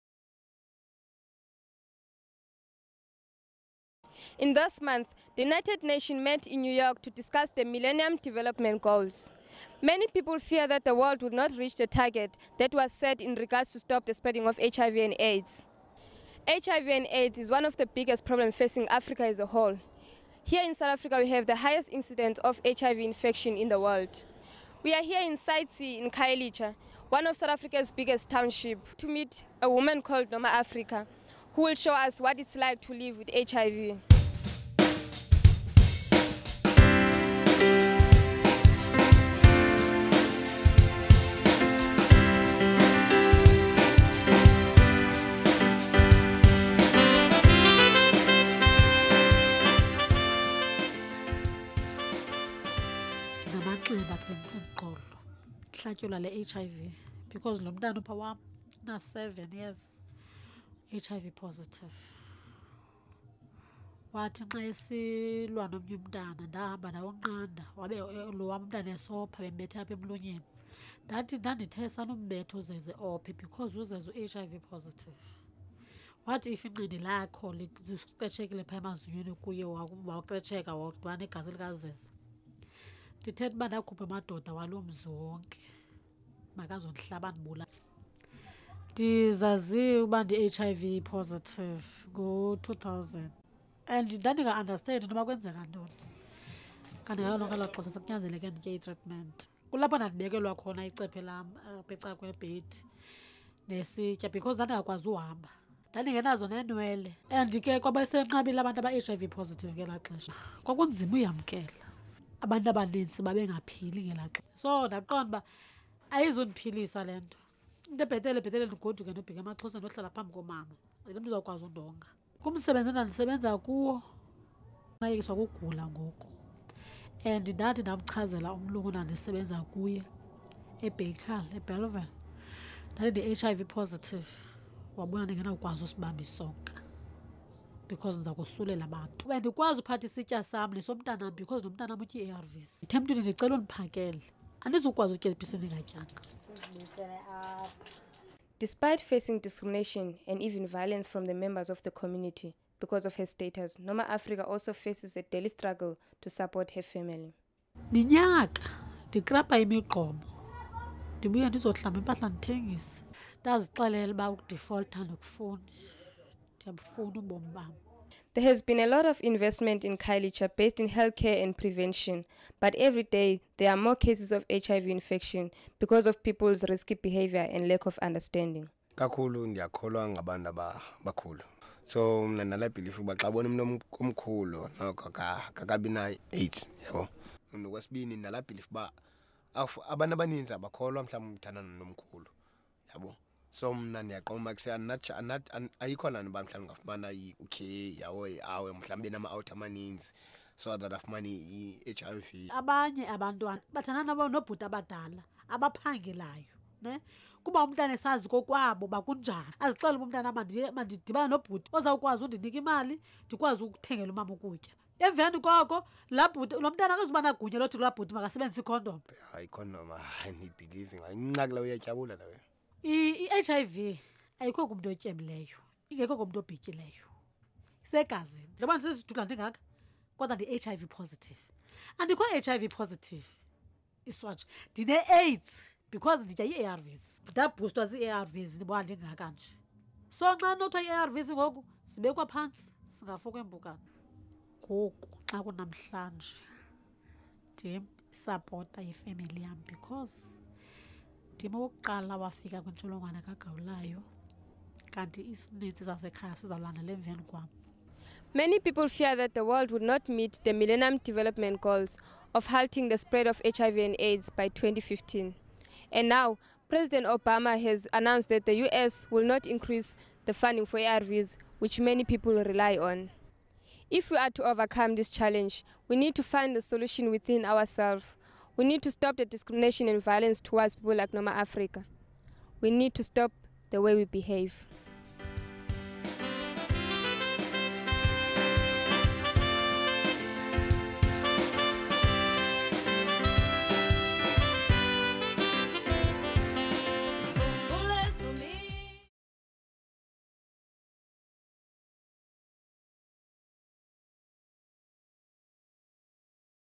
Living with AIDS, 'HIV Cycle in the Township'
This film from the Siyakhona group in Khayelitsha highlights the challenges and discrimination people in the township face once they reveal their HIV status. We are lead by Noma, who after finding out her status in 2000 has come along way to changing her behavious and attitude with her condition.